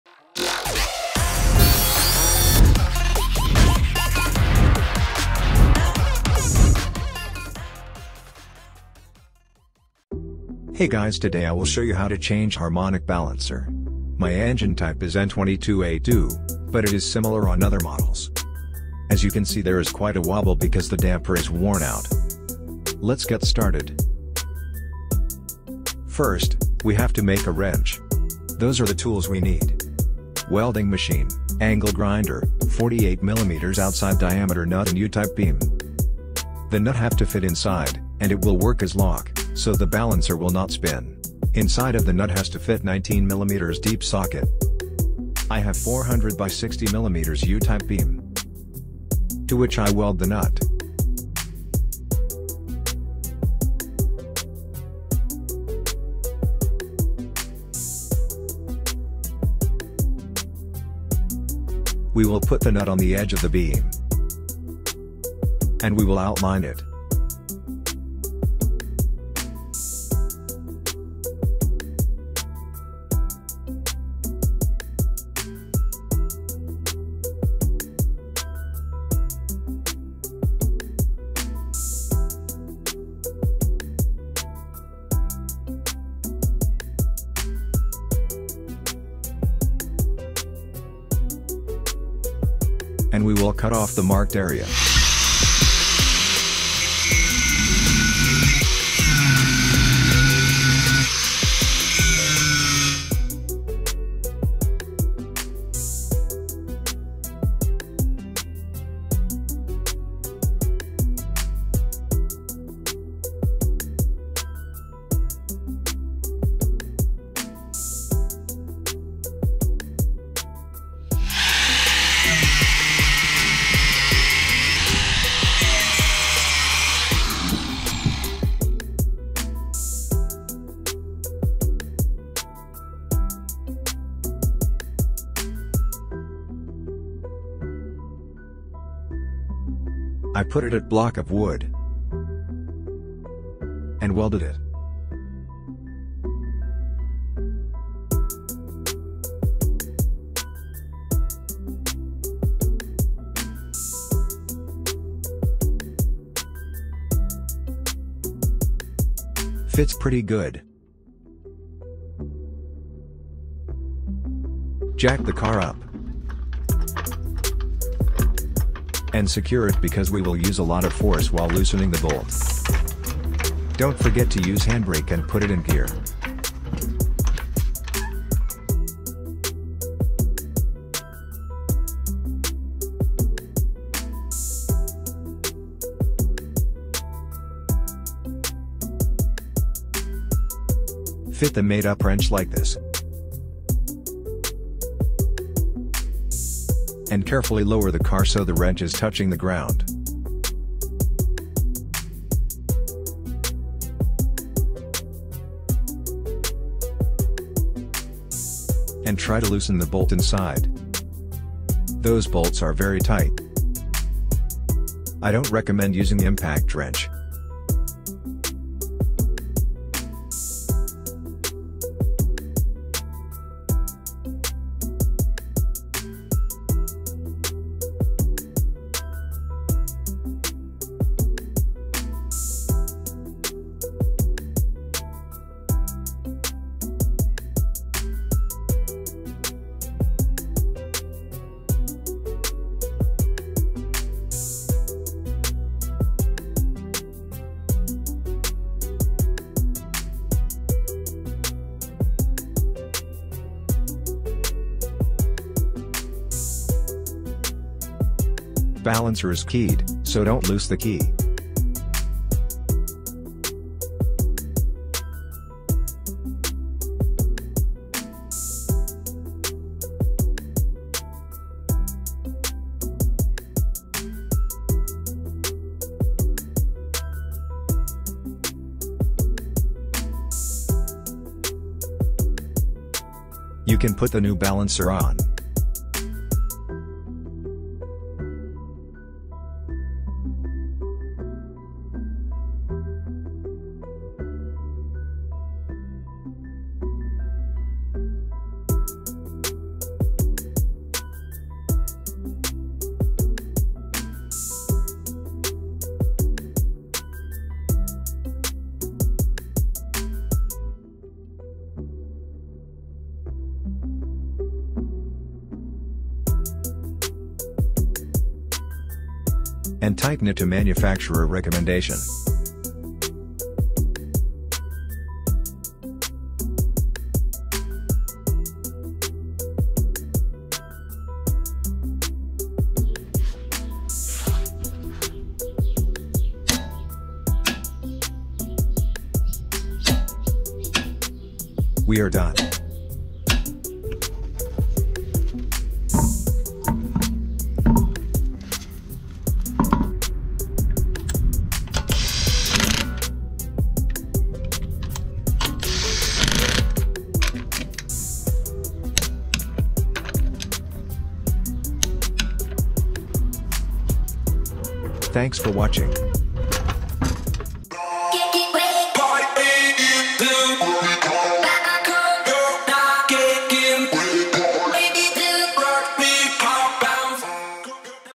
How To Remove Crank Bolts/Pulley + DIY Honda Crankshaft Pulley Holder
How To Remove Crank Bolts/Pulley + Production(DIY) Honda Crankshaft Pulley Holder

Honda Crankshaft Pulley Holder 50mm HEX
13810-RBD-E01
50Mm Harmonic Damper Crankshaft Crank Pulley Wrench Holder Tool Removal Holding Spanner For Honda For Acura Engines

Easy work